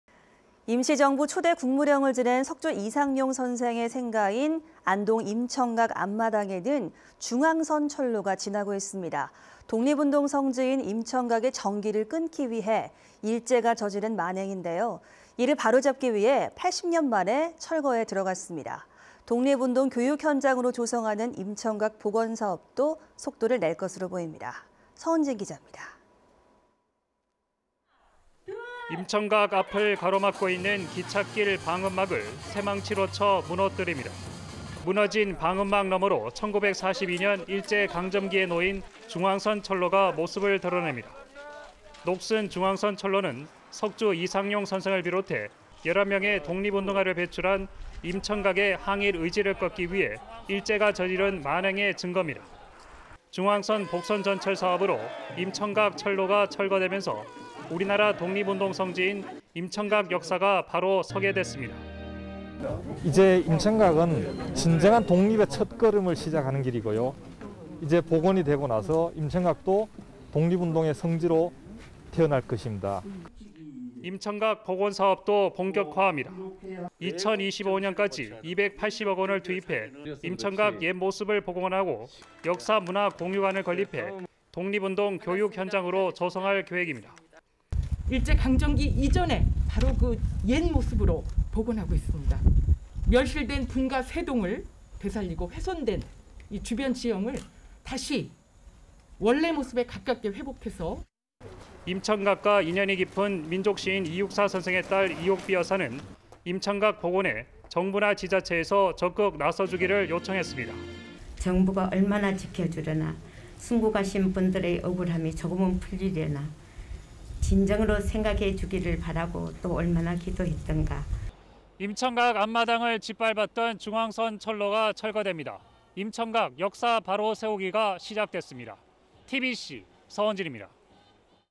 80년 만에 철거 복원 속도 201217 [TBC-띠비띠]
2025년까지 복원, 독립운동 교육 현장 조성